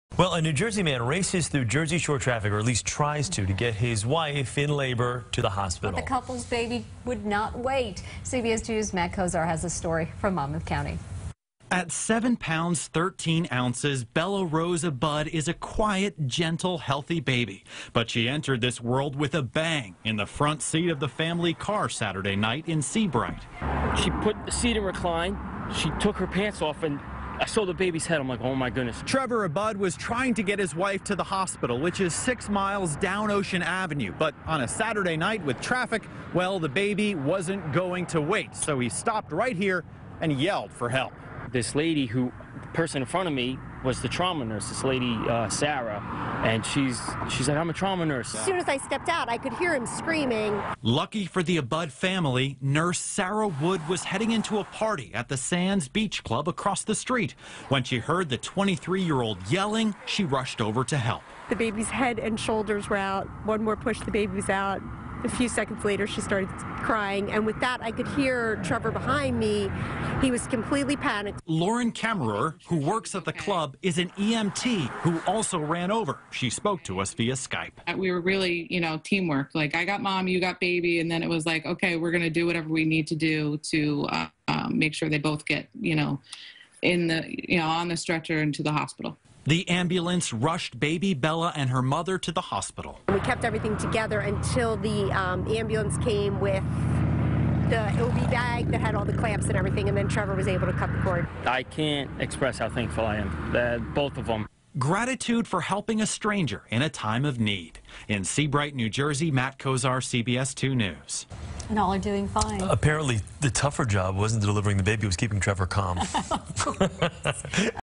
Baby Born On NJ Highway
CBS's Matt Kozar reports.